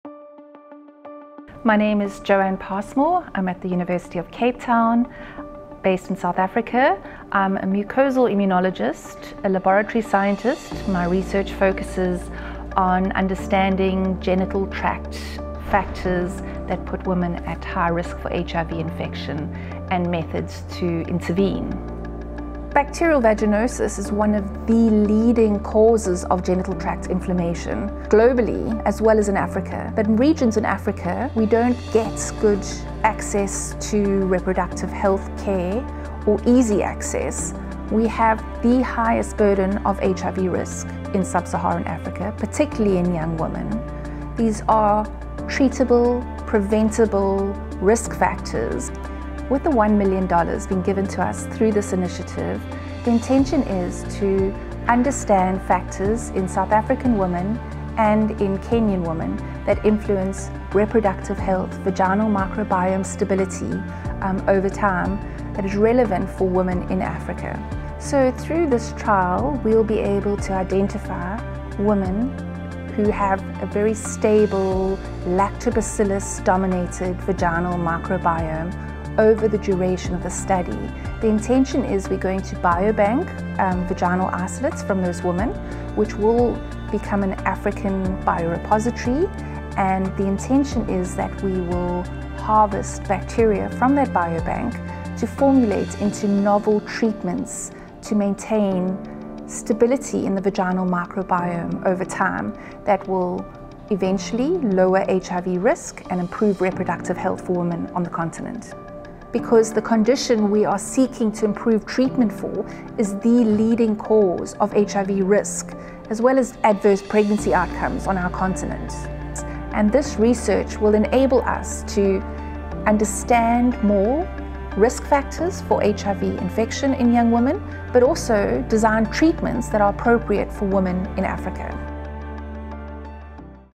Improving sexual reproductive health in women using advanced vagina microbiome research
Jo-Ann Passmore, Associate Professor of the University of Cape Town in South Africa, will pilot the formation of the vaginal microbiome research consortium in Africa (VMRC4Africa) by establishing a network of researchers and centers of excellence for conducting research and clinical trials to promote women’s health across the continent.